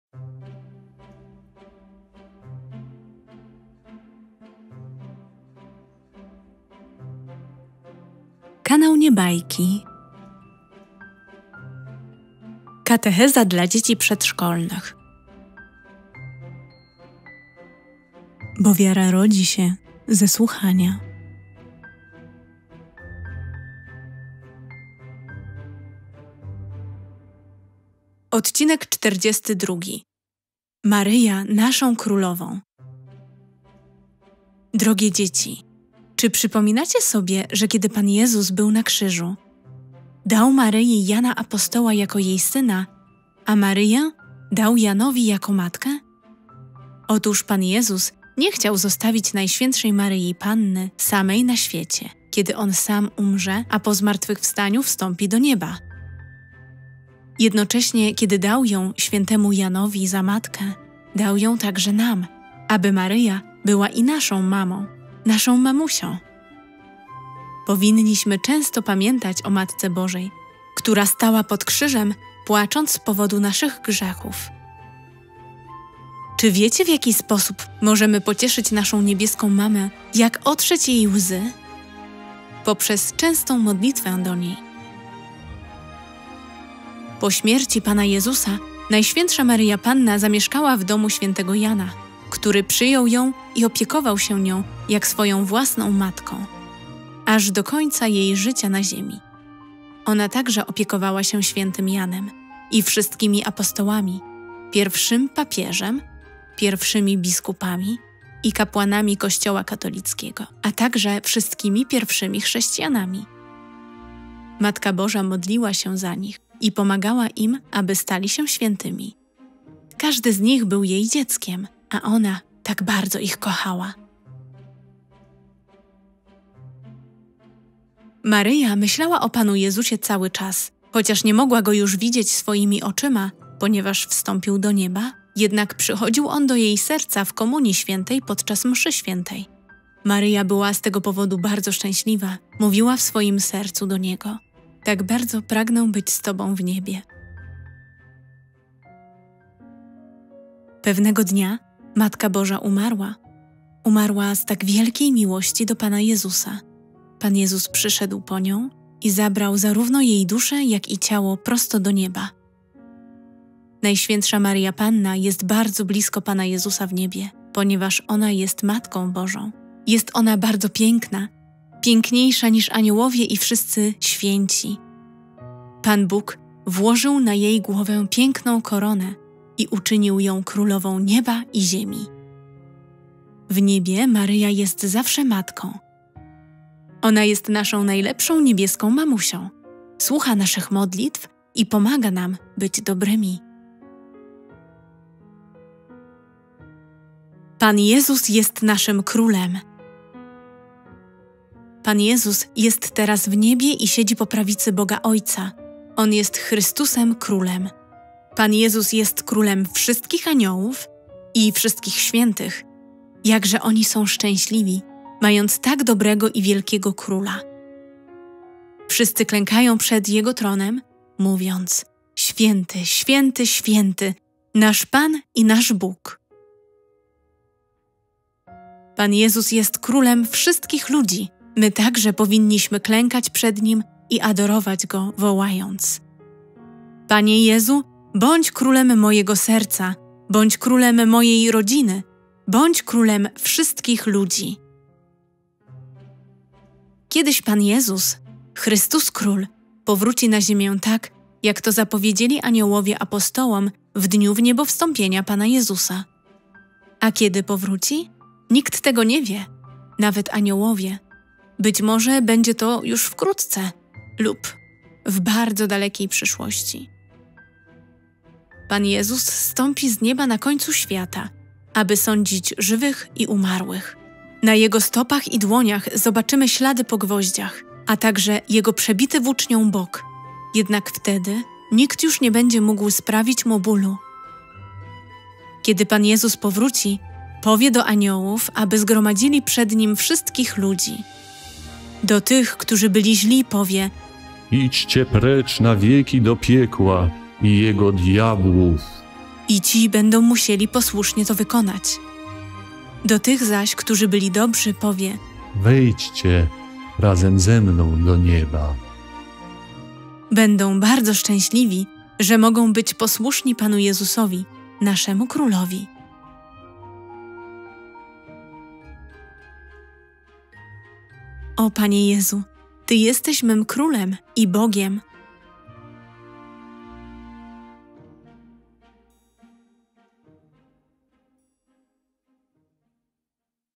KATECHEZA DLA DZIECI PRZEDSZKOLNYCH: #42 - Maryja naszą Królową
Nagrania zostały opracowane na podstawie katechez, które są własnością Sióstr Bractwa św. Piusa X. SFSPX Saint Michel en Brenne France Mars 2001, ISBN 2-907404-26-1